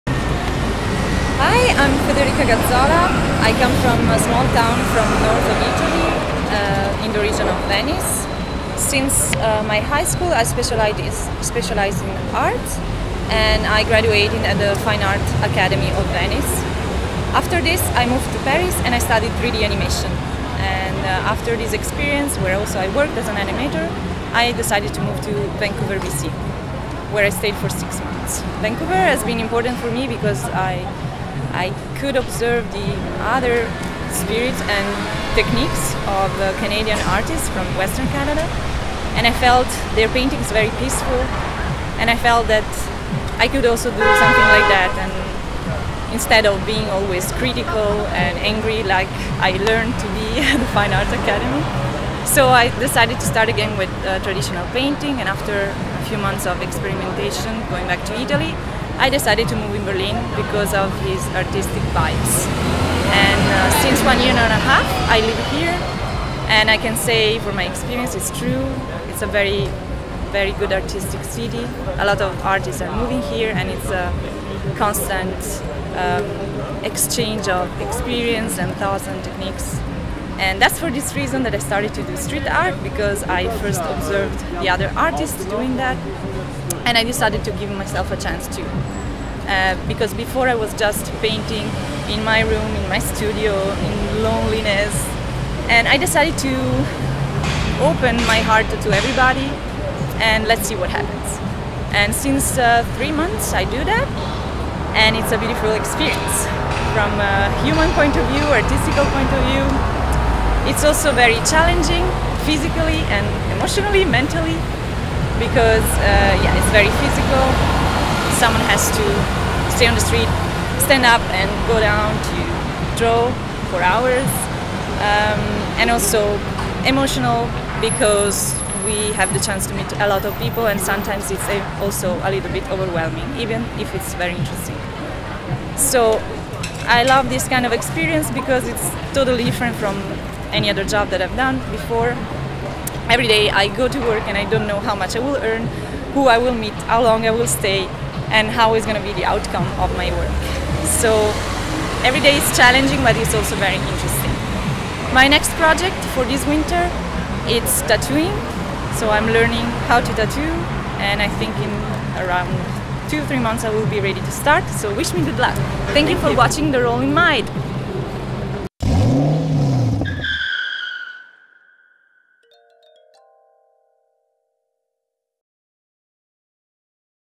STREET ART GERMANY - Federica Gazzola - INTERVIEW
Mit Kreide und Kohle zeichnet „Federica Gazzola“ faszinierende Kunstwerke auf den grauen Beton und verschönert die Straßen Berlins.